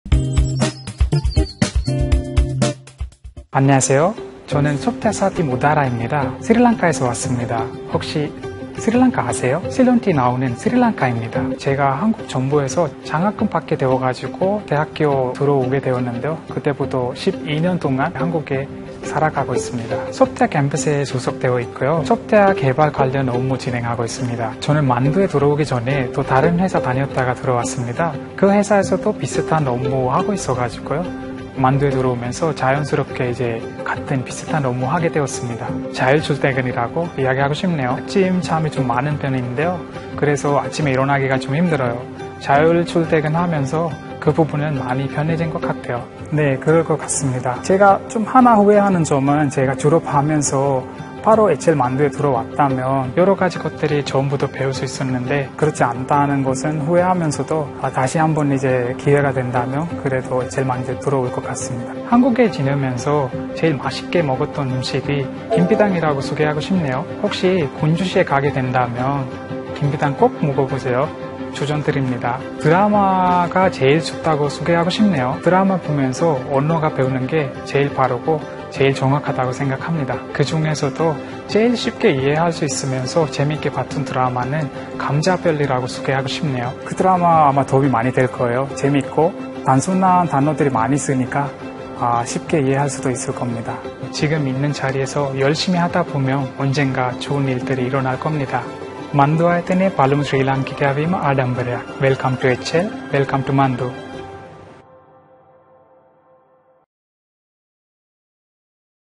[Welcome to HL] EP.1 한국살이 12년차 스리랑카 출신 우다라 연구원이 전하는 HL만도 회사생활!
HL에서 함께 꿈을 키워나가고 있는
글로벌 직원들의 이야기를 들어보는 [Welcome to HL] !

첫번째 편으로 SW Campus 소속 Udara 연구원님과 함께했습니다!
HL만도에서 일하게 된 계기와 함께
한국에서 경험했던 재밌는 이야기들을 들어볼까요?

*한국인보다 더 능숙한 한국어 실력도 확인해보세요!